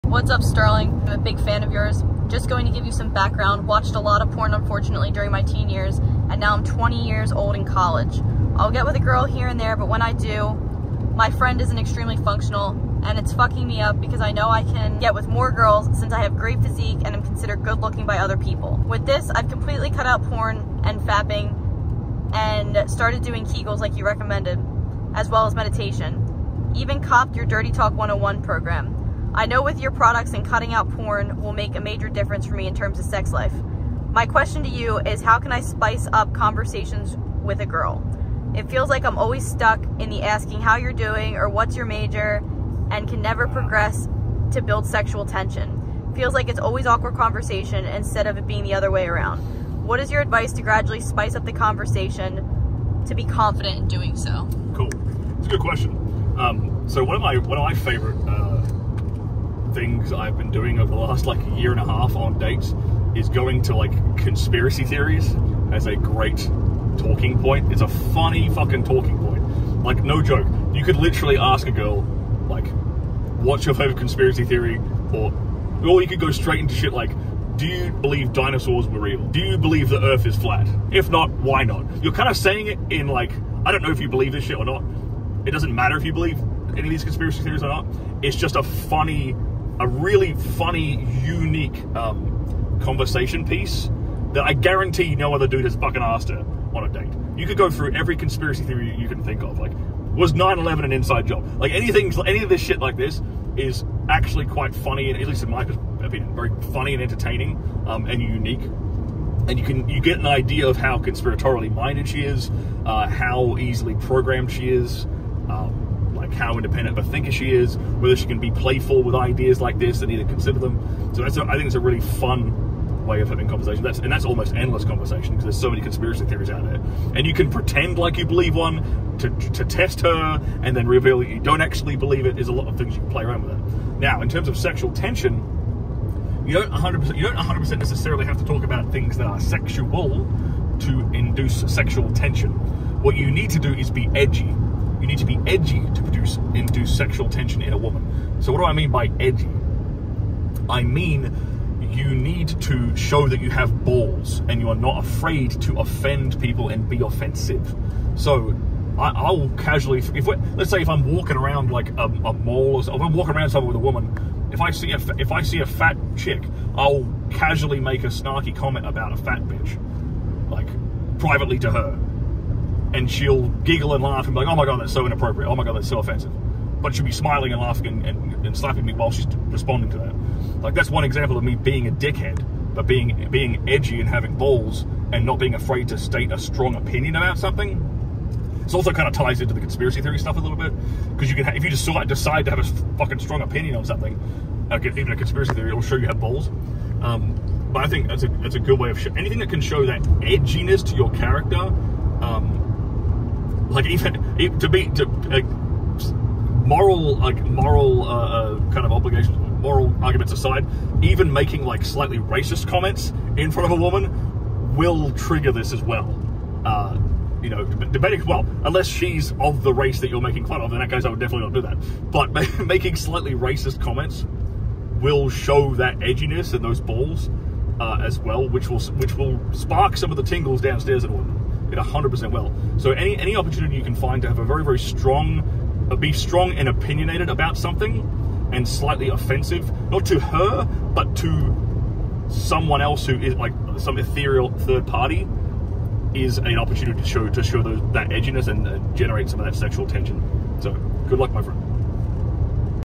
Sexual Tension: How to Spice Up Conversation with a Girl | Stirling Cooper AMA
How to create sexual tension and spice up conversation with a girl. Sex coach, Stirling Cooper, gives advice.

The advice Stirling gives revolves around being edgy, showing confidence, and taking unconventional conversation approaches like discussing conspiracy theories and expressing strong opinions.

Stirling suggests engaging in controversial topics or making slightly offensive comments in a playful manner to create intrigue and generate sexual tension. The emphasis is on displaying boldness and willingness to challenge social norms to make interactions more interesting and memorable. The goal is to evoke emotions and stimulate attraction through unconventional conversational tactics.

This video is a transcript of an Ask Me Anything (AMA) session with sex coach, Stirling Cooper.

A woman reads out the short back-story provided by the correspondent, followed by his specific question, which Stirling then responds to with an answer and advice.

In this AMA the correspondent is Johnson J. from the US.

💊 Last Longer - Prevent Premature Ejaculation
💊 Male Enhancement - Rock Hard Bloodflow
💊 Libido Booster - Go Multiple Rounds Recover Fast

Timestamps:

0:00  Client: How to spice up conversation with a girl?
0:34  How to build sexual tension?
0:55  Stirling: Conspiracy theories
2:14  Sexual tension
2:23  Be "edgy"

CS240322PV1